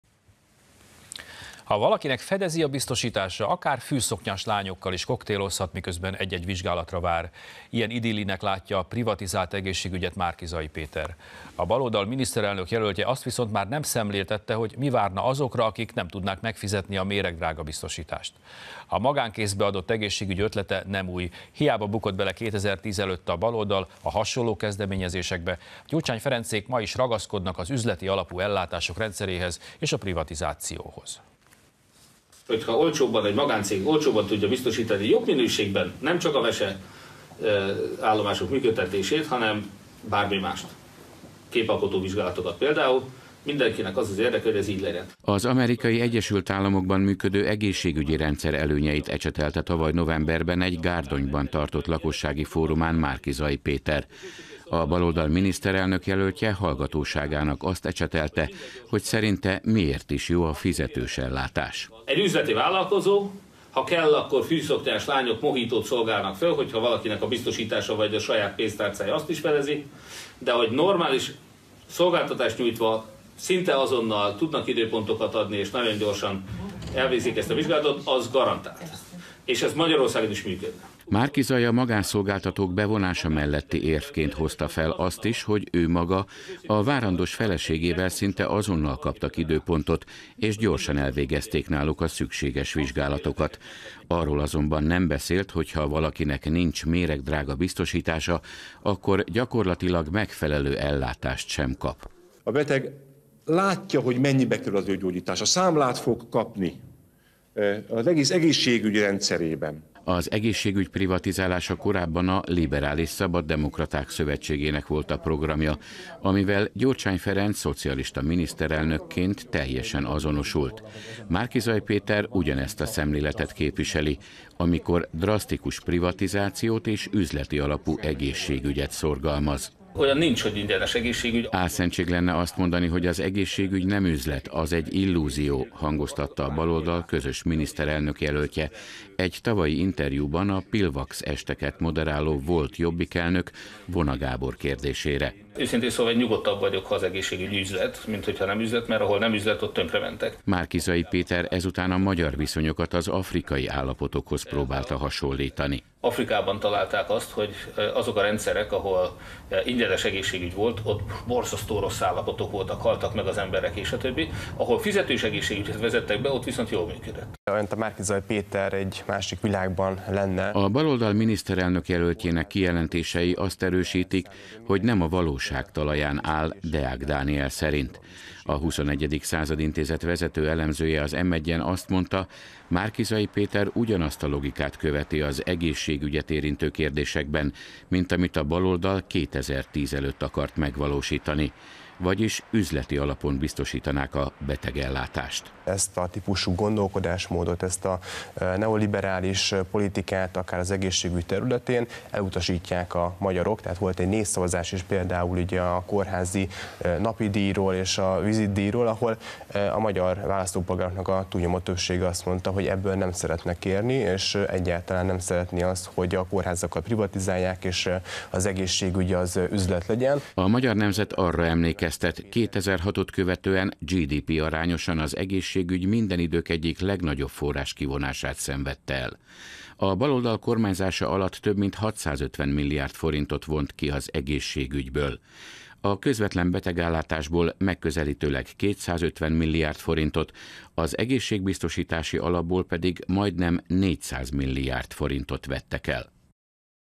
Magánegészségügyben hisz a baloldal
Ha valakinek fedezi a biztosítása, akár fűszoknyás lányokkal is koktélozhat, miközben egy-egy vizsgálatra vár – ilyen idillinek látja a privatizált egészségügyet Márki-Zay Péter. A baloldal miniszterelnök jelöltje azt viszont már nem szemléltette, hogy mi várna azokra, akik nem tudnák megfizetni a méregdrága biztosítást. A magánkézbe adott egészségügy ötlete nem új, hiába bukott bele 2010 előtt a baloldal a hasonló kezdeményezésekbe. Gyurcsány Ferencék ma is ragaszkodnak az üzleti alapú ellátások rendszeréhez és a privatizációhoz.